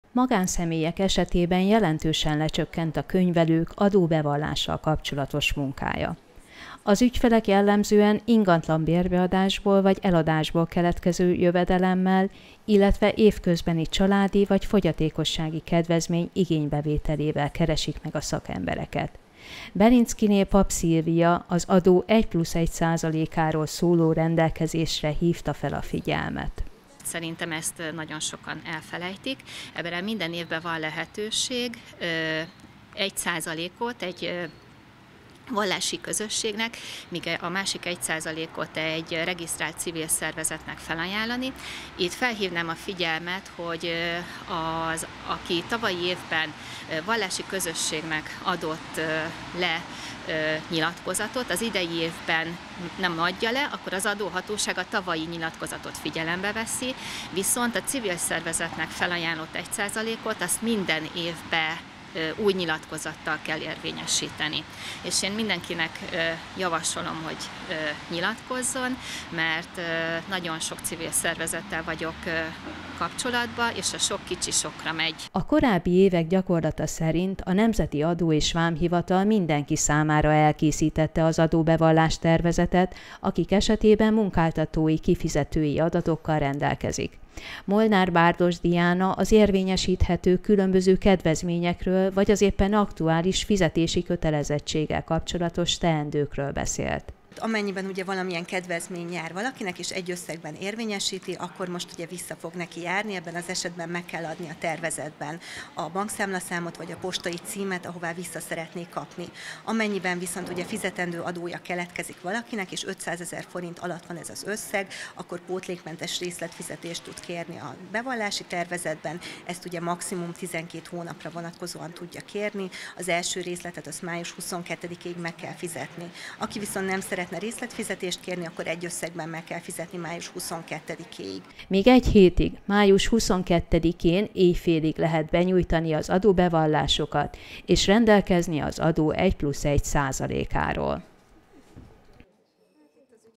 Közeleg az adóbevallási határidő vége
Magánszemélyek esetében jelentősen lecsökkent a könyvelők adóbevallással kapcsolatos munkája. Az ügyfelek jellemzően ingatlan bérbeadásból vagy eladásból keletkező jövedelemmel, illetve évközbeni családi- vagy fogyatékossági kedvezmény igénybevételével keresik meg a szakembereket. Belinckyné Papp Szilvia az adó 1+1 %-áról szóló rendelkezésre hívta fel figyelmet.

A korábbi évek gyakorlata szerint a Nemzeti Adó- és Vámhivatal mindenki számára elkészítette az adóbevallás-tervezetet, akik esetében munkáltatói kifizetői adatokkal rendelkezik. Molnár-Bárdos Diána az érvényesíthető különböző kedvezményekről vagy az éppen aktuális fizetési kötelezettséggel kapcsolatos teendőkről beszélt.

Még egy hétig, május 22-én, éjfélig lehet benyújtani az adóbevallásokat, és rendelkezni az adó 1+1 %-áról.